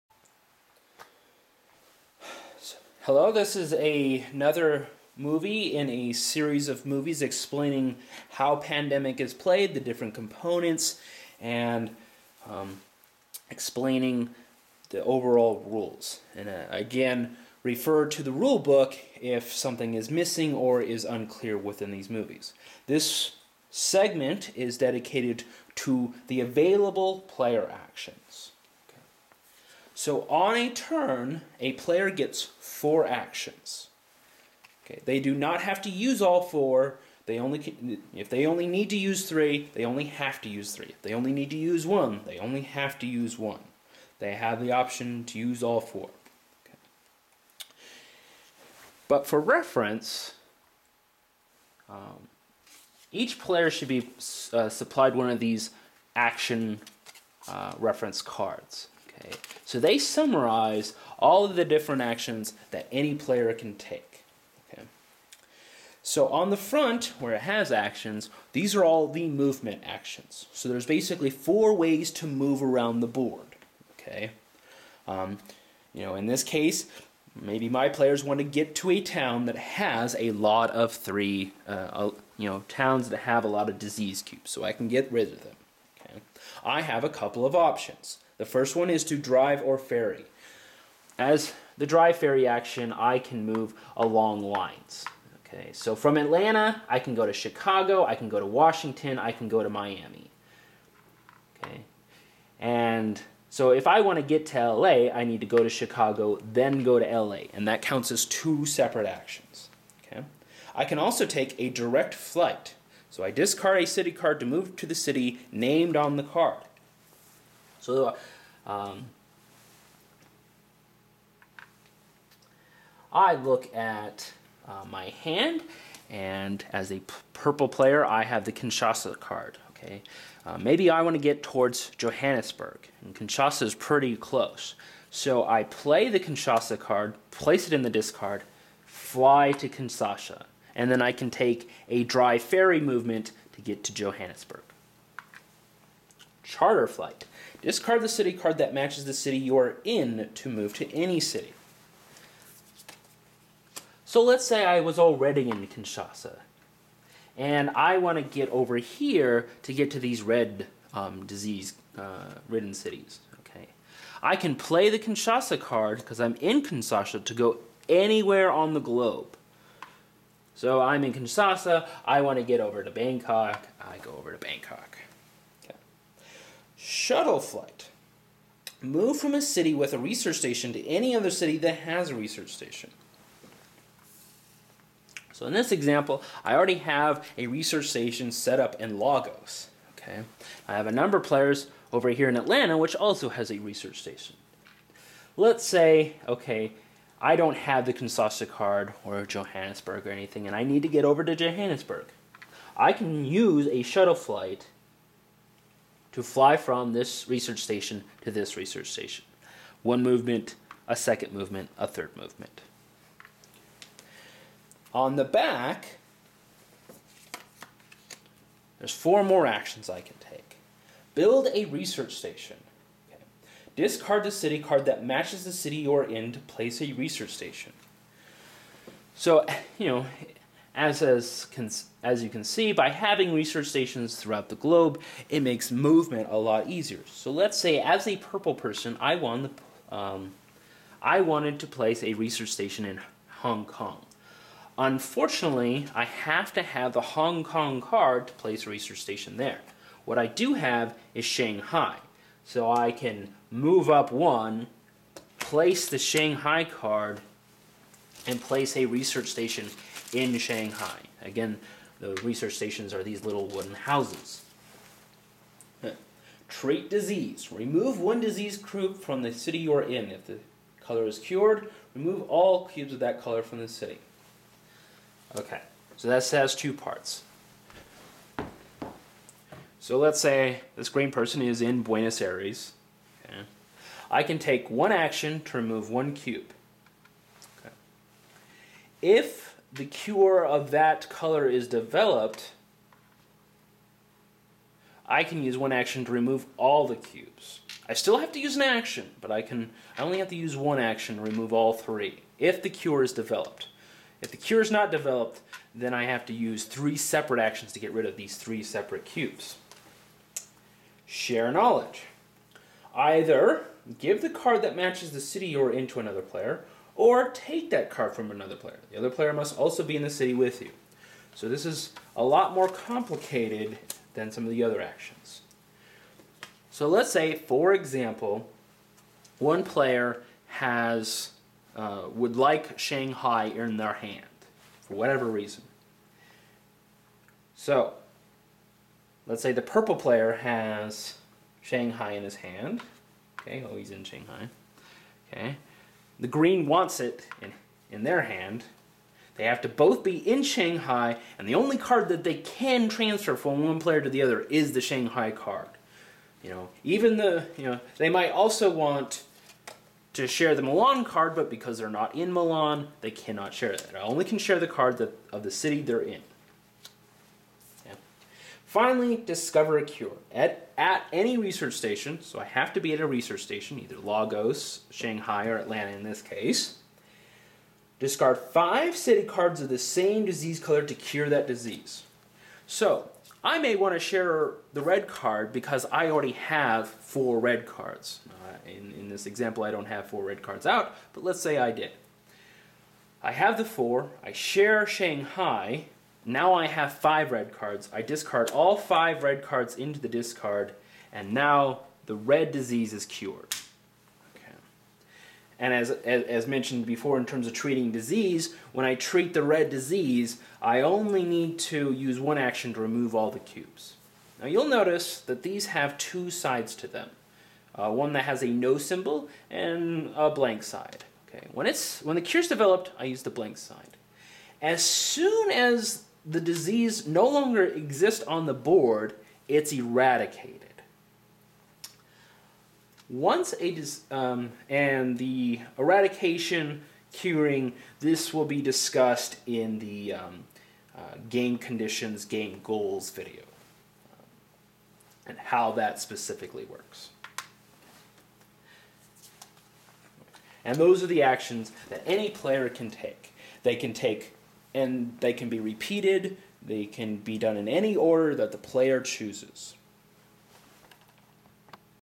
Pandemic Overview of Rules (2 of 5) - available player actions
This is a summary of the 8 different actions you can take to progress yourself towards the goal of preventing and controlling disease spread and developing four cures.